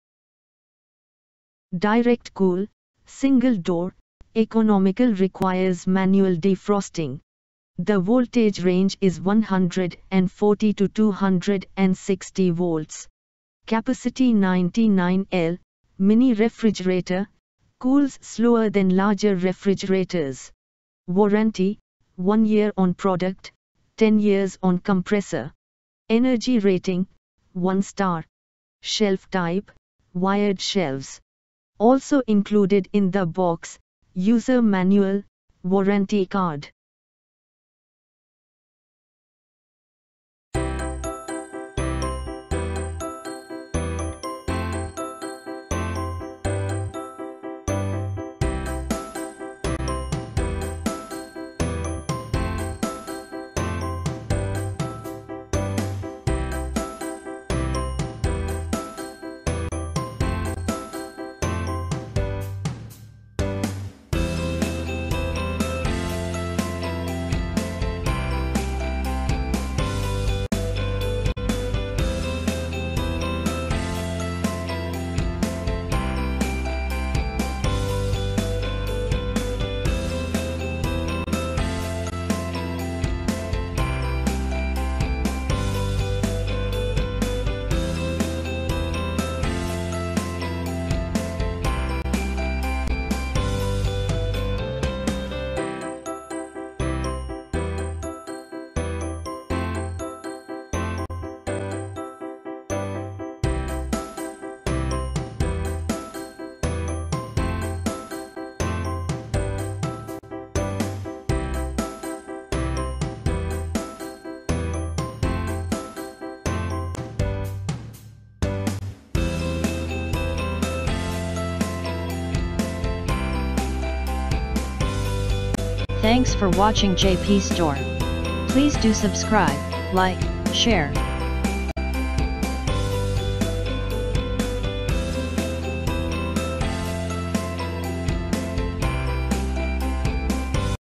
Godrej 99L 1 Star Direct Cool Single Door Refrigerator (RD CHAMP 114 WRF 1.2 WIN RED, Wine Red)
Direct Cool, Single Door: Economical requires manual defrosting. The voltage range is 140-260 volts
Capacity 99 L: Mini Refrigerator, Cools slower than larger refrigerators
Warranty: 1 year on product, 10 years on compressor
Energy Rating: 1 Star
Shelf Type: Wired Shelves
Also included in the box: User manual, Warranty card